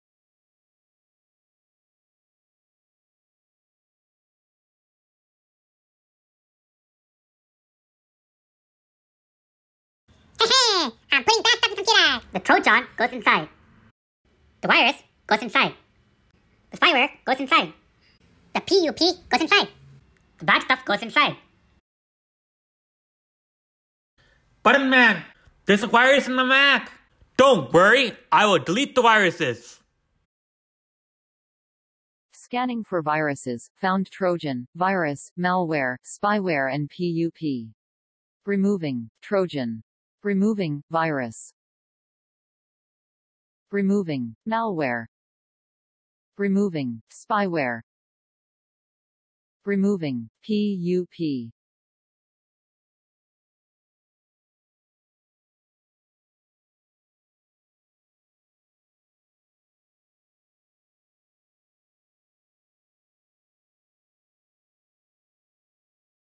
Button Man Final Movie
This video is about Button Man getting and defeating viruses